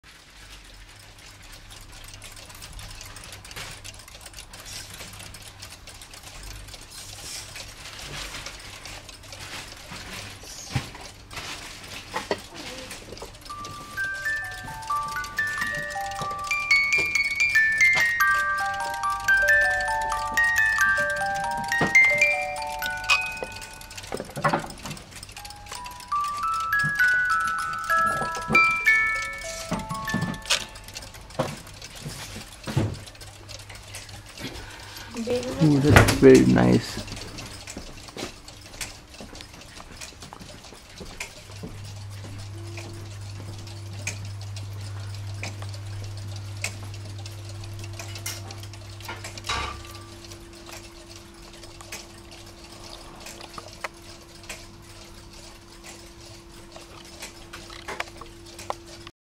Watch beautiful cuckoo clock sound effect - from Interlaken, Switzerland
Watch the beautiful cuckoo clock sound effect in the heart of Interlaken, Switzerland.. Cuckoo clock shop visit in Interlaken is a must in your Europe travel, especially when passing through Switzerland. They are simply the most beautiful clocks, adding beauty on any wall